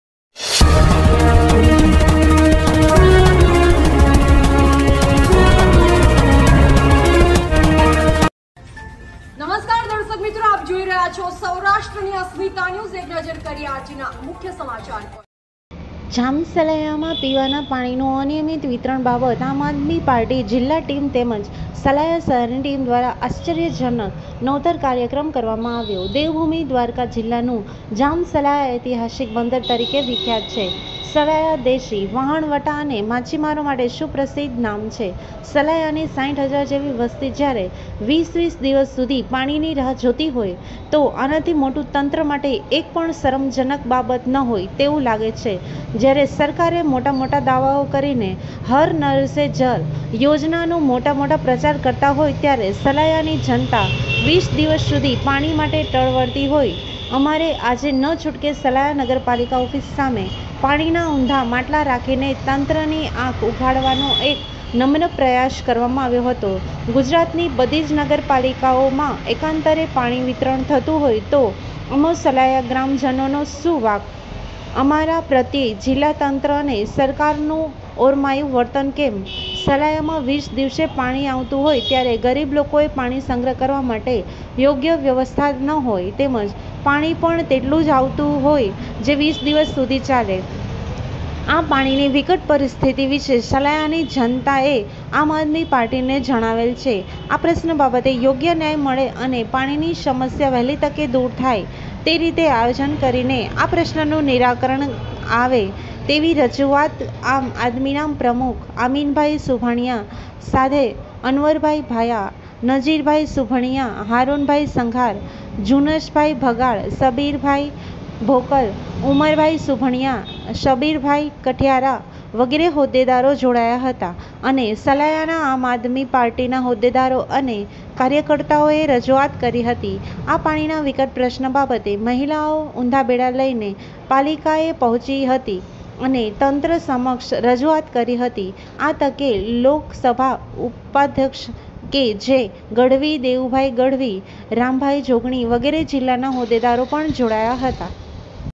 જામસલાયા માં પીવા ના પાણી નું અનિયમિત વિતરણ નથતા આમ આદમી પાર્ટી ના કાર્યકરો ઉત્તરીયા મેદાને
જામ સલાયા માં પીવા ના પાણી નું અનિયમિત વિતરણ બાબત આમ આદમી પાર્ટી જિલ્લા ટીમ તેમજ સલાયા શહેર ની ટીમ દ્વારા આશ્ચર્ય જનક (માટલા ઊંધા રાખીને ) નવતર કાર્યક્રમ કરવામાં આવ્યો.